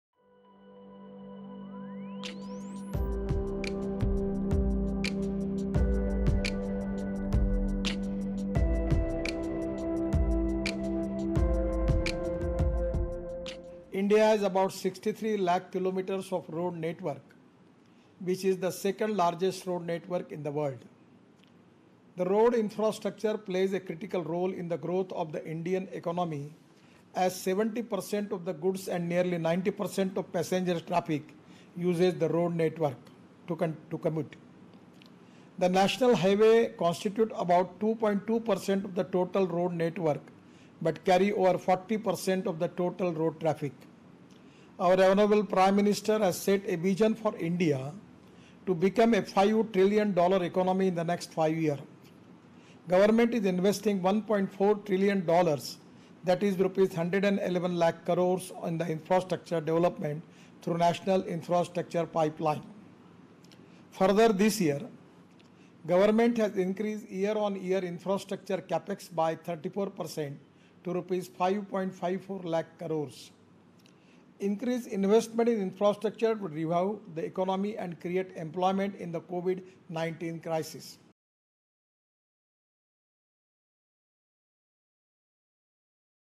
Road infrastructure plays critical role in growth of Indian economy: Gadkari
Union Minister for Road Transport & Highways, Nitin Gadkari on July 09 addressed the 16th annual conference on 'Road Development in India', via video conferencing. Addressing a virtual event, Gadkari said the Central government is investing 1.4 trillion dollars (Rs 111 lakh crores) in infrastructure development through National Infrastructure Pipeline (NIP).

Courtesy: ANI